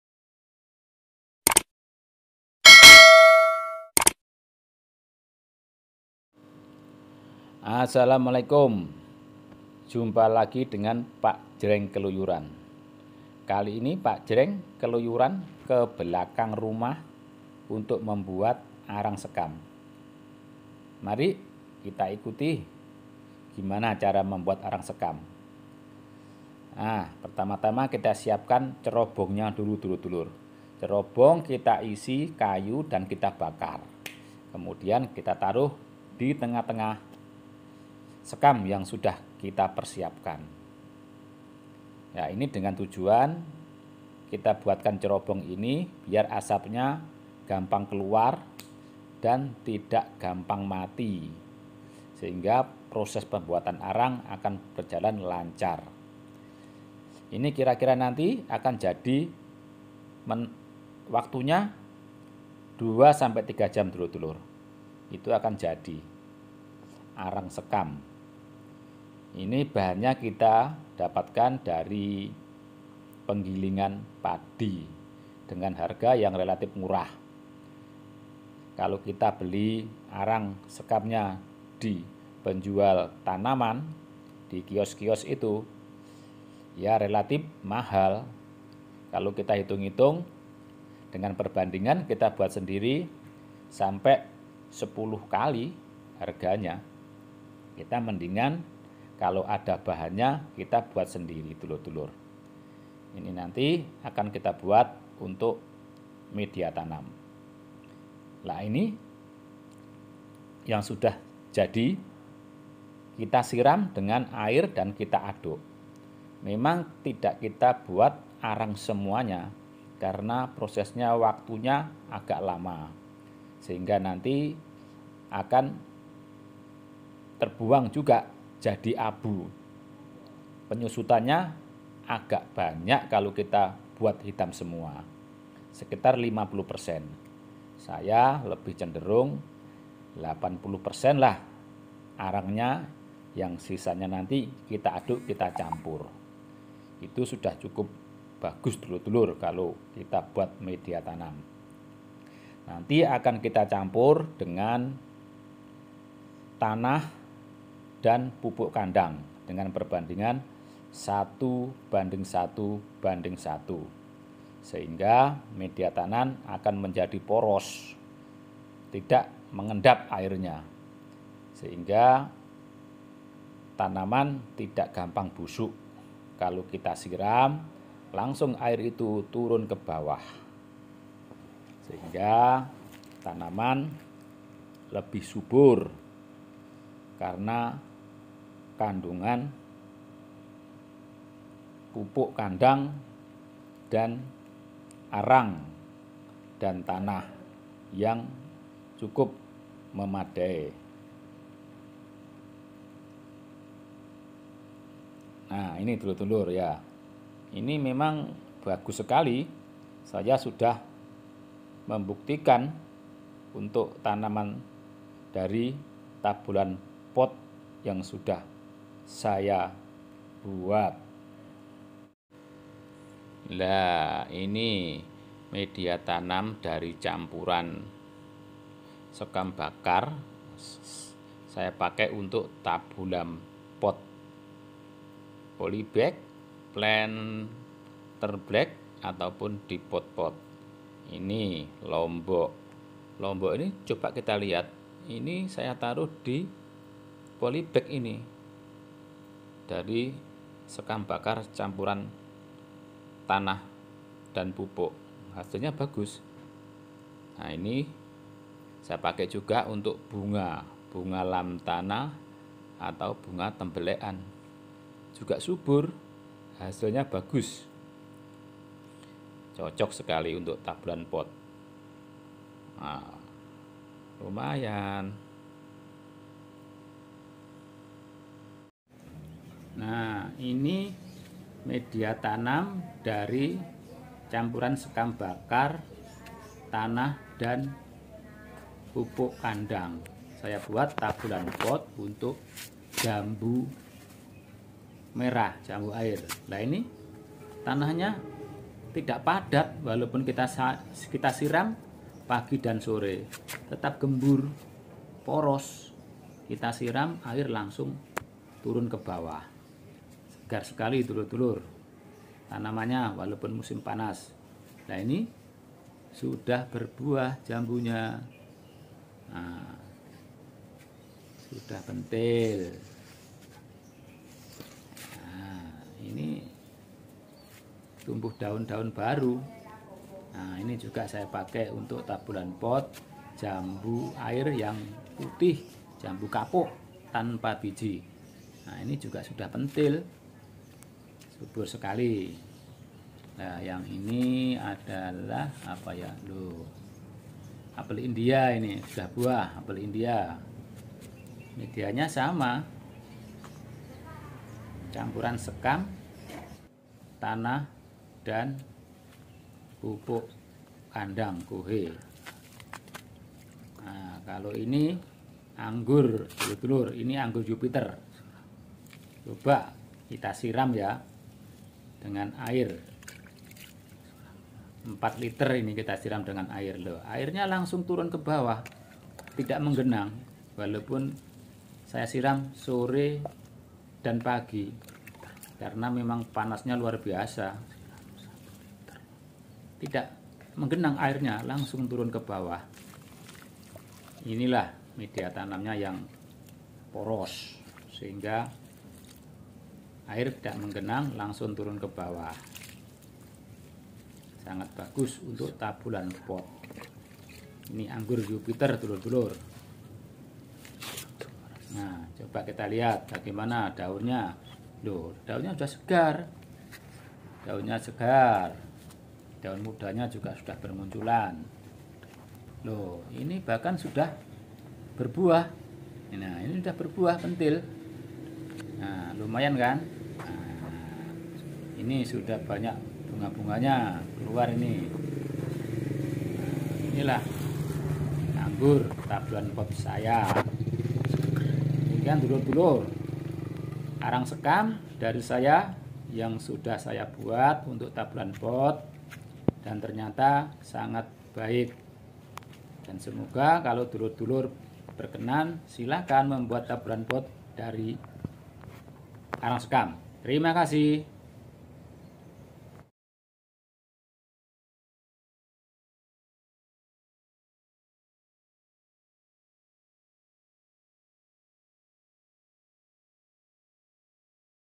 Buat Arang Sekam Untuk Tabulampot
Assalamualaikum
Buat arang sekam untuk tabulampot ini dimagsudkan untuk menekan beaya untuk media tanam tabulampot ,karena bahan dari sekam padi harga yang murah kita olah menjadi arang sekam padi atau arang sekam bakar  ini bisa kita pergunakan untuk media tanam dengan mencampurnya dengan tanah dan rabuk kohe dengan perbandingan 1:1:1 ,media ini bisa juga dipakai dydroponik yaitu murni arang sekam saja,dengan komposisi tersebut media taman penjadi poros tidak mudah padat/mengeras jika disiram air langsung kebawah/keluar tidak tidak menggenang yang bisa membuat busuknya akar tanaman.Demikian semoga bermanfaaf dan sampai bertemu kembali di video selanjutnya.